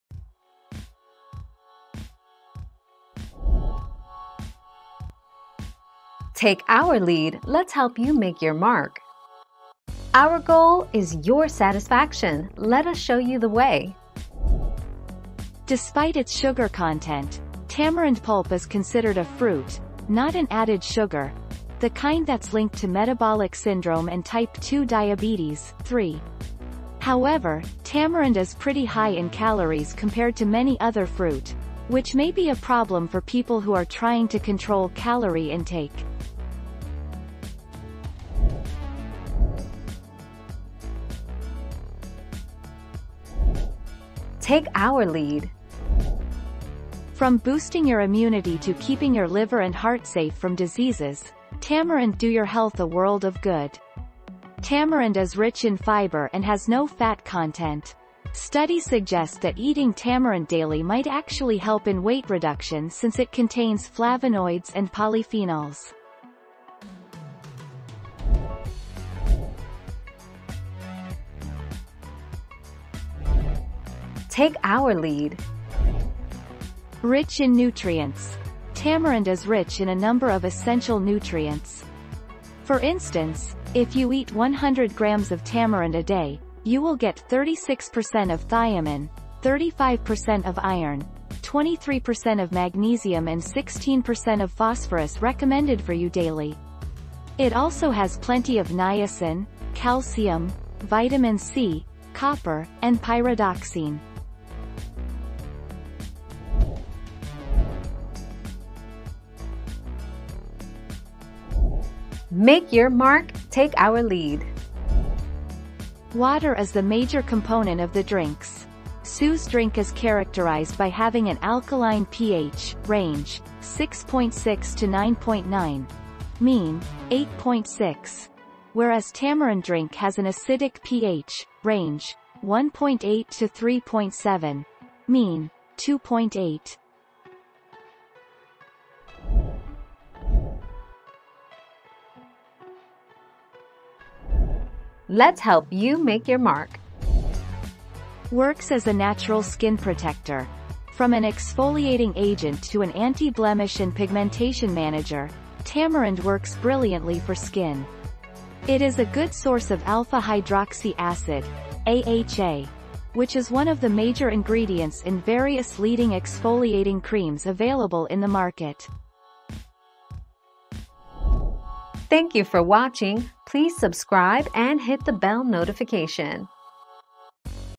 Why tamarind is not good for health?
00:00 - Why tamarind is not good for health?
00:40 - Can I eat tamarind everyday?
01:10 - What happens if you eat tamarind everyday?
01:48 - Is Tamarind alkaline or acidic?
02:20 - Is Tamarind good for skin?

Laura S. Harris (2021, May 8.) Why tamarind is not good for health?
    AskAbout.video/articles/Why-tamarind-is-not-good-for-health-242308